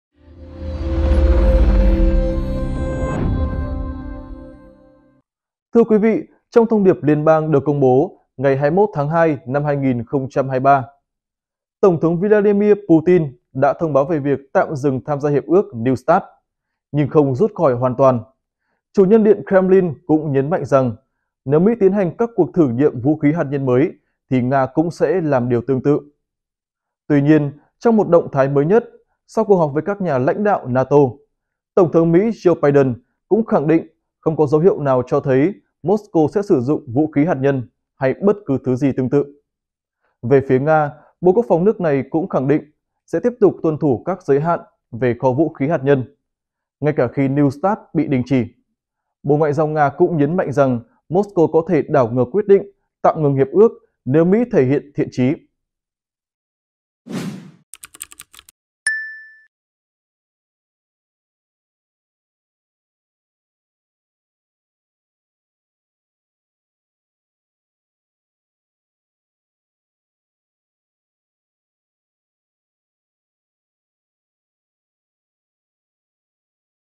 Động thái mới nhất của tổng thống Mỹ sau khi Nga đình chỉ hiệp ước NEW START | Vietnamnet
Trong một động thái mới nhất sau cuộc họp với các nhà lãnh đạo NATO, Tổng thống Mỹ Joe Biden (Giâu Bai đừn) khẳng định không có dấu hiệu nào cho thấy Moscow sẽ sử dụng vũ khí hạt nhân hay bất cứ thứ gì tương tự.